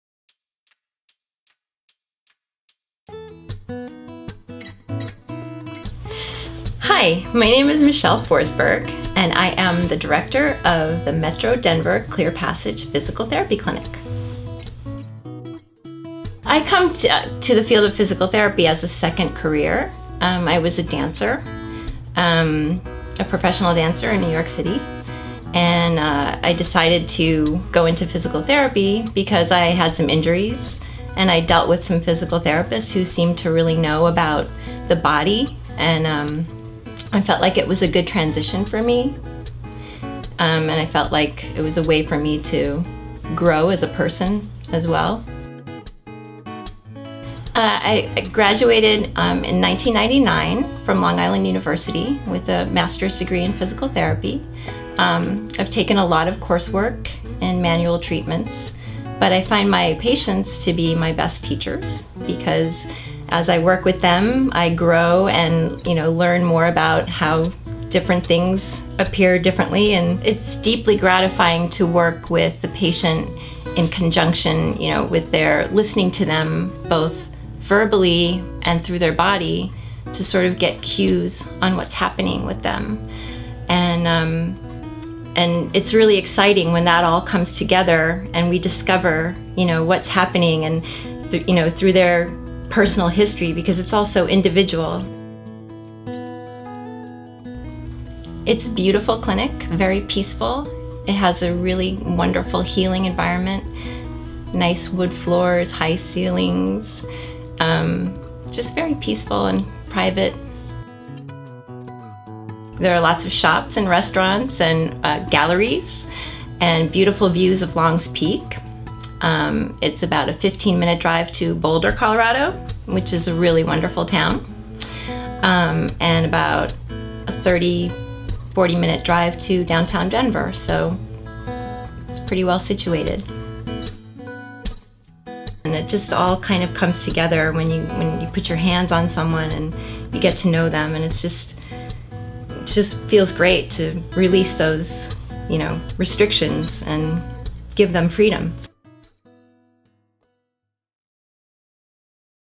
Meet the Therapist: Michele Forsberg, PT
Michele Forsberg, PT, MS is Clinic Director of our Metro Denver, Colorado location. A former dancer, Michele graduated magna cum laude from the University of the Arts in Philadelphia.; She went on to perform professionally in New York City and parlayed her knowledge and appreciation of the body and movement into a career in fitness. Always striving to learn more, she enrolled at Long Island University where she earned her Master's Degree in Physical Therapy in 1999 and has spent the early half of her career in New York City running her exclusive personal training and physical therapy practice.

Hi my name is Michele Forsberg, and I am the director of the Metro Denver Clear Passage Physical Therapy Clinic.

I come to the field of physical therapy as a second career. I was a dancer.--A profession al dancer in New York City. I decided to go into physical therapy, because I had some injuries. I dealt with some physical therapists that seemed to really know about the body. I felt like it was a good transition for me, and I felt like it was a good way for me to grow as a person, as well.

I graduated in 1999 from Long Island University with a Master's degree in physical therapy. I've taken a lot of coursework in manual treatments, but I find my patients to be my bet teachers. As I work with them, I grow and learn more about how different things appear different. It's deeply gratifying to work with a patient. In conjunction with listening to them both verbally and through their body to sort of get cues as to what is happening with them. It is really exciting when all that comes together and we discover what sharpening through their personal history because you know it's all so individual.

It is a beautiful clinic, very peaceful it has a really healing environment; nice wood floors and high ceilings.

There are lots of shops and restaurants and beautiful views of Longs Peak. It is a about a 15 minute drive to Boulder, Colorado, which is a really wonderful town and about a 30-40 minute drive to downtown Denver. So, it's pretty well situated

It all comes together when you put your hands on someone and you get to know them. It just feels great to release those restrictions and give them freedom